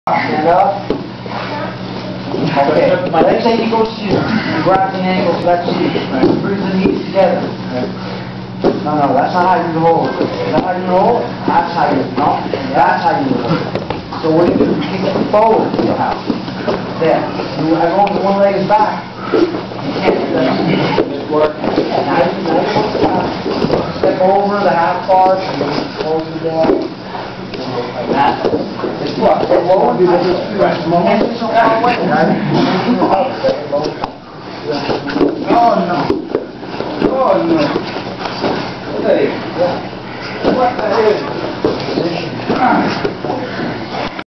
Renzo Gracie Jiu Jitsu Academy training session 3/6/09 Weston
max and peter